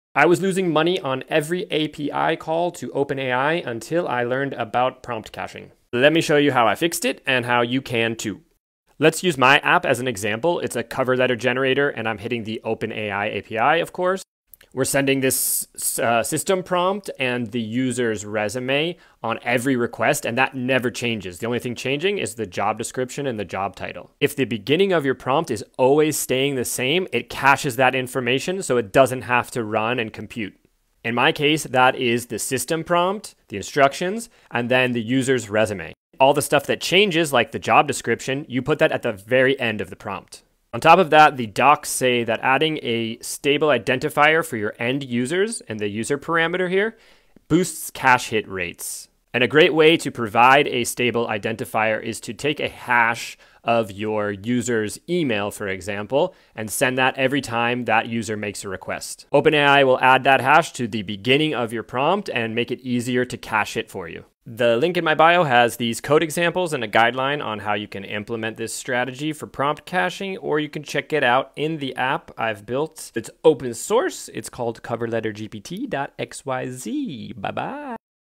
80% faster and cheaper OpenAI API via Prompt Caching.
I made my gpt wrapper 80% faster and cheaper by structuring my prompts for OpenAI api prompt caching.